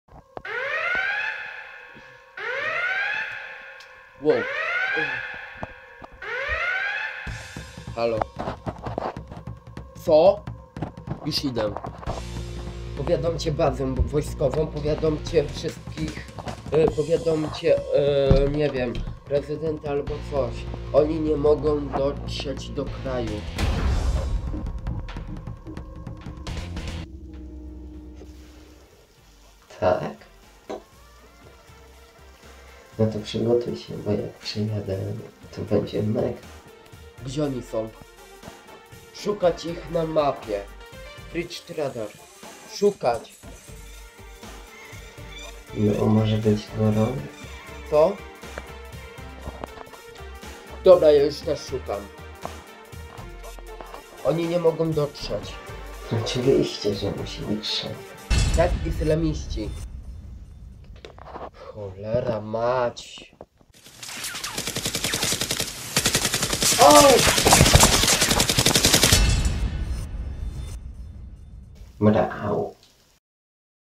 KAPITAN SZWED
TRAILER FILMU W STYLU JAMES BOND

W ROLI GŁÓWNEJ SEBASTIAN SZWED "KIDRAMAN"

Agent dostaje telefon z wiadomością że islamiści porwali samolot aby zaatakować Polskie miasto, idzie szybko namierzać ich a mapie gdy po chwili zostaje zaatakowany.